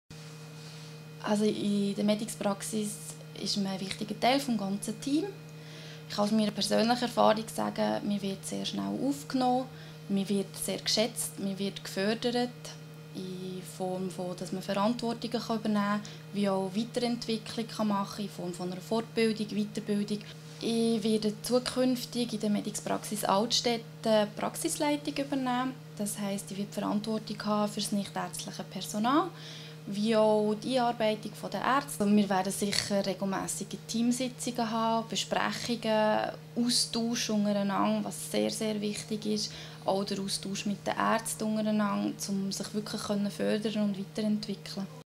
Als MPA bei mediX kann ich mich weiterentwickeln
Am 2. Mai 2016 eröffnet mediX direkt beim Bahnhof Altstetten in Zürich eine weitere Praxis. Was diese den dort den MPAs bietet, sagt die künftige Praxisleiterin Sylvia Kost in ihrem Statement zur neuen Praxis.